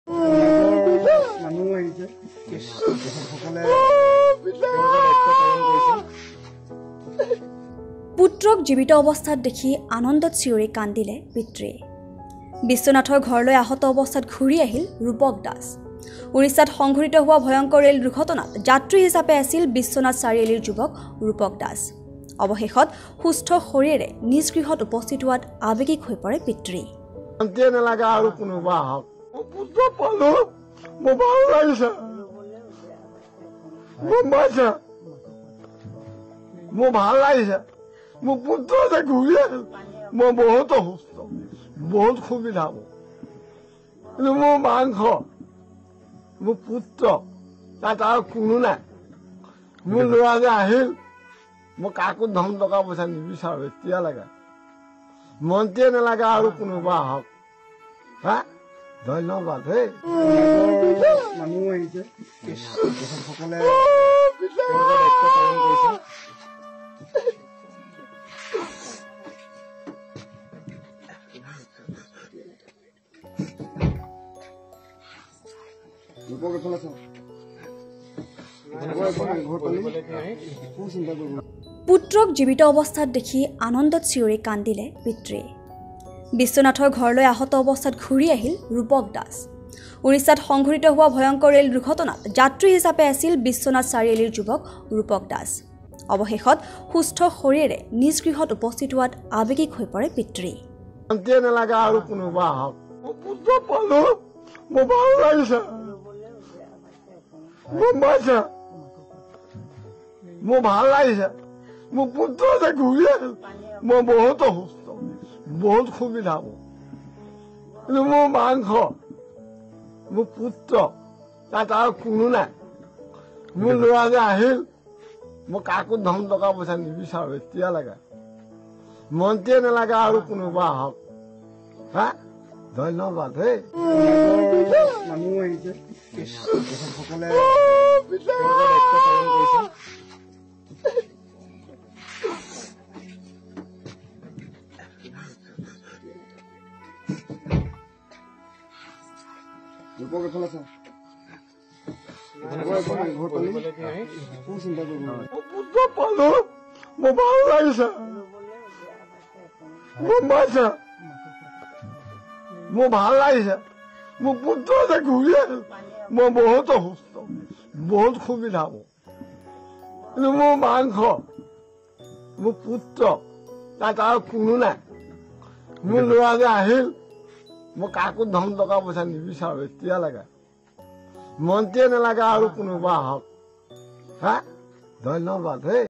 পুত্ৰক জীৱিত অৱস্থাত দেখি আনন্দত চিঞৰি কান্দিলে পিতৃয়ে | Orissa Train Accident
🛑 "মই পুত্ৰক পালো, মোক মন্ত্ৰীৰ ধন টকা পইচা নালাগে"
🛑 পুত্ৰক জীৱিত অৱস্থাত দেখি আনন্দত চিঞৰি কান্দিলে পিতৃয়ে
🛑 উৰিষ্যাৰ ৰেল দুৰ্ঘটনাত পতিত হৈছিল একমাত্ৰ পুত্ৰ ৰূপক দাস